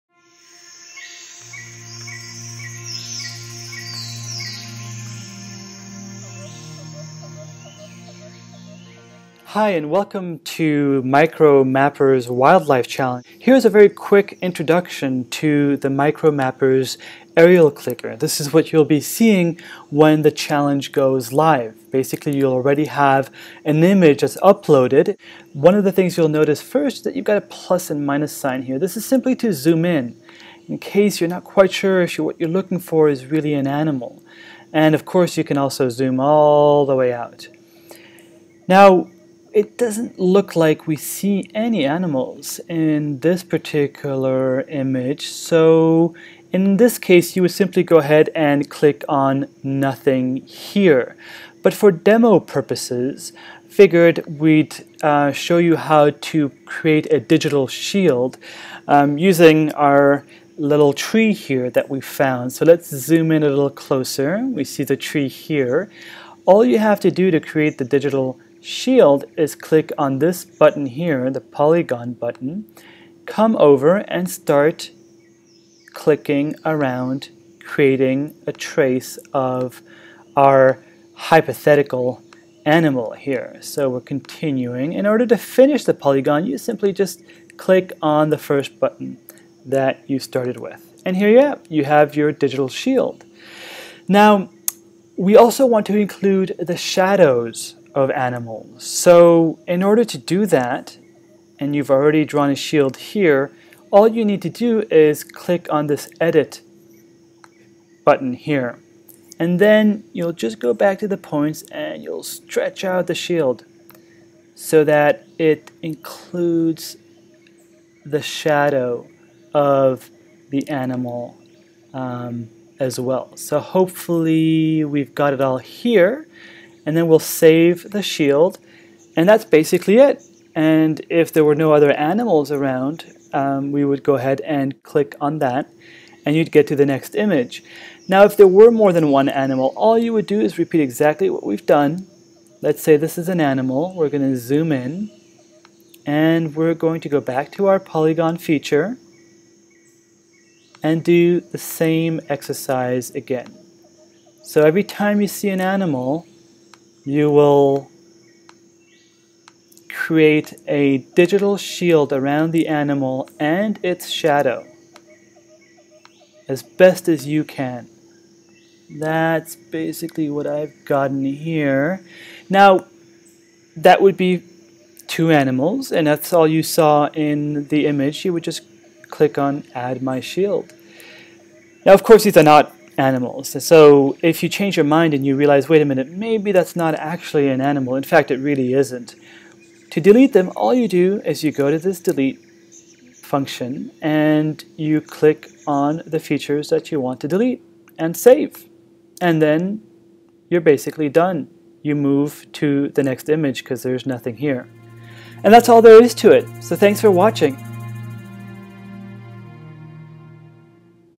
MicroMappers Wildlife Challenge in Namibia
Here's a short tutorial on how to use the MicroMappers Aerial Clicker to search for wild animals in Namibia in high resolution aerial images taken using UAVs. This initiative is part of a wildlife monitoring and protection project with Kuzikus Wildlife Research in Namibia. Please see MicroMappers.org for more information.